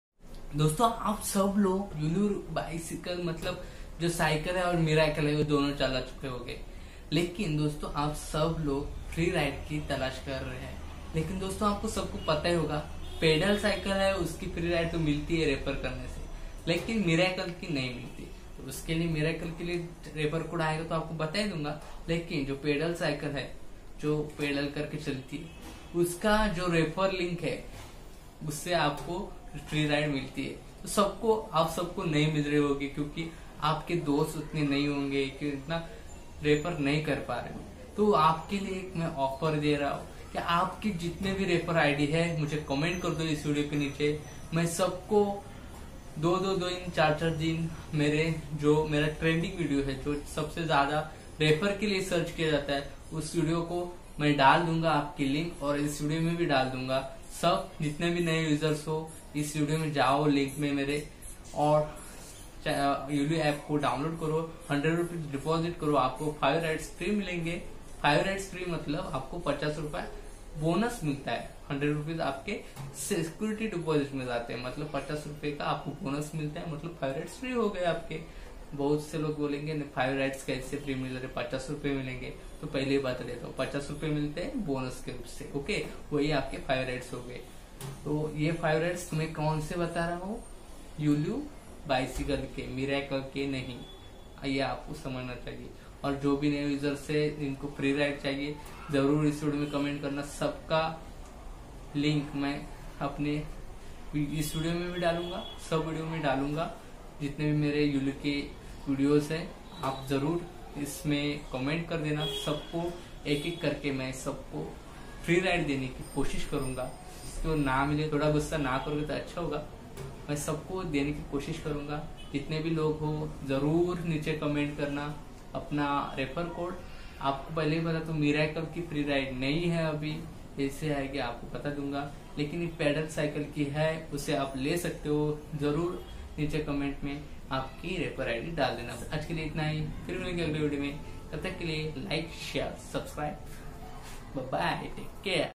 Yulu bycycle ride free unlimited || yulu bicycle free || yulu cycle free
This video help you to get unlimited free rides from me and free to use yulu bicycle unlimited time

Yulu refer code:Sign up with Yulu and get one free ride!

yulu refer code
yulu bicycle refer code
yulu ride free
yulu unlimited free
yule free code